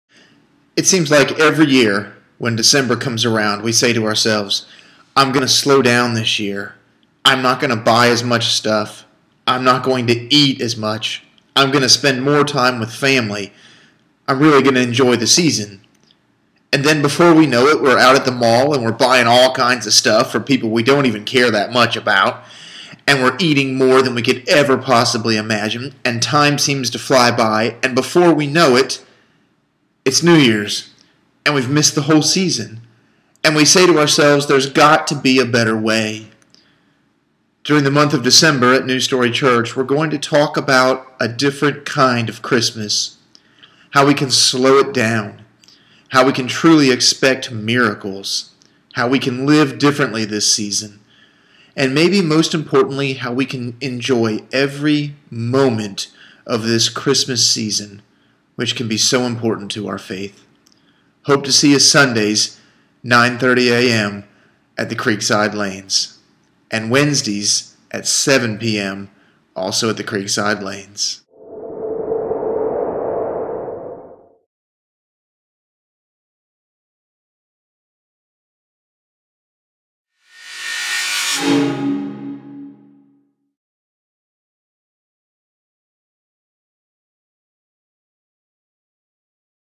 A Different Kind of Christmas
This Christmas season at New Story Church we'll talk about how we can truly enjoy the Christmas season - without all the overbuying, overeating, and overbusying ourselves.  Come join us as we learn how it's okay not to be perfect, how to expect miracles, and more.  Sunday mornings at 9:30 a.m. at the Creekside Bowling Lanes in Winston Salem.